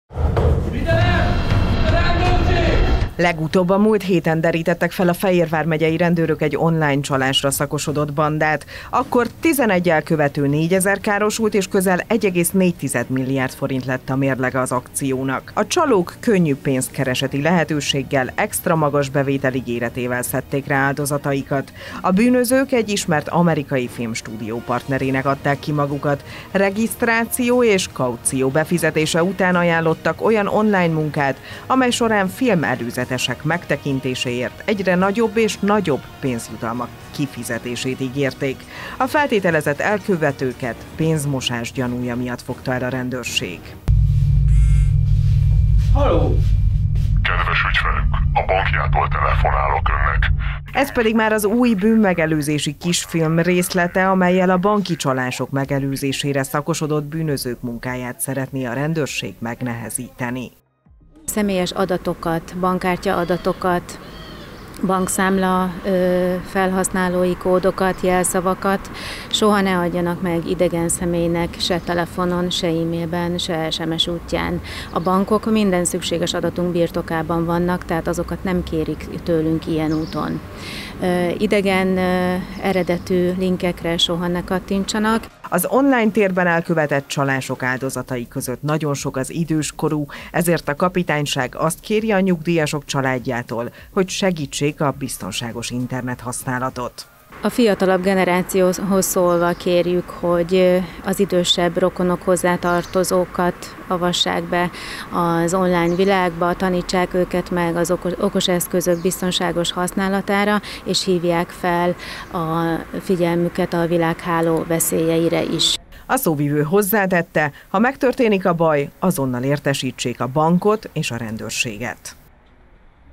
Videó az online csalások ellen
Egyre több bűncselekmény történik az online térben. A rendőrség most egy videót tett közzé annak érdekében , hogy csökkentse az áldozatok számát. A vármegyei kapitányság szakemberei arra hívják fel a figyelmet, hogy a bankszámla adatokat ne adja meg senki telefonon, e-mailen és sms-en keresztül sem.